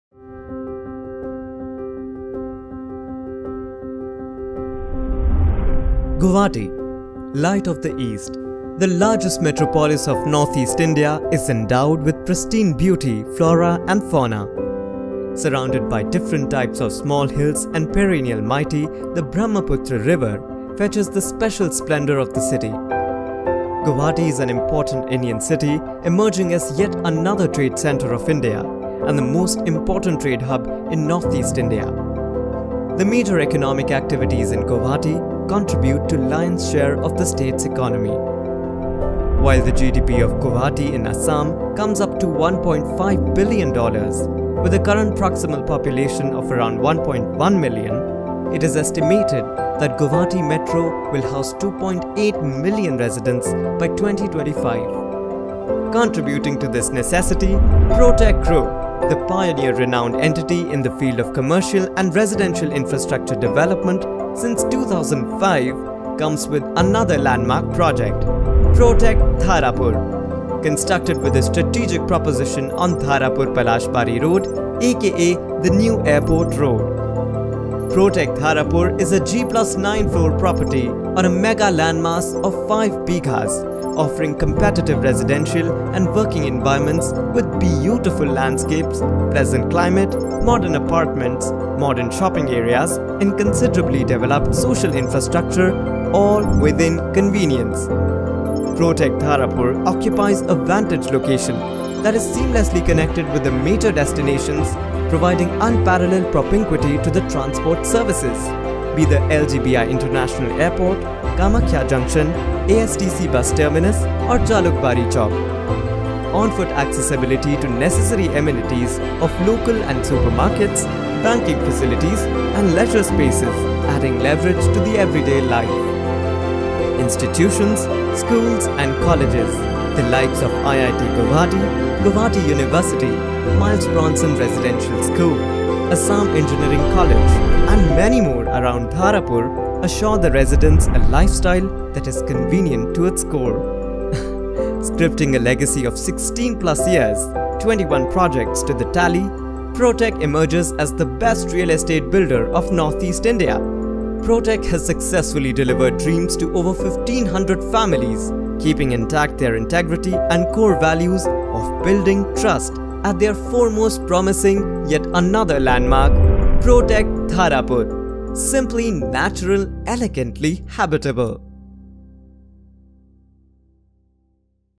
Area Wiki | Protech Dharapur | 3 BHK Residential Apartments | Guwahati | Protech Group
Protech Dharapur occupies a vantage location that is seamlessly connected with the major destinations of Guwahati. The transport facilities, the necessary amenities and leisure spaces around Dharapur assures the residents a lifestyle that is convenient to its core.

Nearby landmarks

Educational Institutions
1. Scamewo Global School - 4.7
2. kmKendriya Vidyalaya AFS - 5.0 km
3. St. Joseph School - 5.2 km
4. Miles Bronson Residential School - 5.2 km
5. Paschim Guwahati Mahavidyalaya - 0.4 km
6. Girijananda Chowdhury Institute of Management & Technology - 2.1 km
7. Assam Don Bosco University - 3.8 km
8. Eastern Valley College - 8.4 km

Hospitals
1. Azara PHC - 2.2 km

Markets
1. Lakhimi Supermarket - 0.5 km
2. Daily Bazar - 0.5 km
3. Easyday Store - 0.5 km

Banks
1. State Bank of India - 0.5 km
2. Canara Bank - 0.5 km
3. United Bank of India - 0.5 km
4. Indian Bank - 0.5 km
5. Assam Gramin Vikas Bank - 0.5 km

A T Ms
1. State Bank of India - 0.5 km
2. Canara Bank - 0.5 km
3. Indian Bank - 0.5 km
4. United Bank - 0.5 km

Petrol Pumps
1. Pradeep Oil - 1.0 km
2. BPCL Petrol Pump - 0.5 km
3. Nayan Filling Station - 1.0 km

Police Stations
1. Azara Police Station - 4.1 km
2. Jalukbari Police Station - 6.0 km
3. Car WashGood Car Wash - 0.5 km

Cinema Hall
1. Pragjyoti Cinema Hall - 10.0 km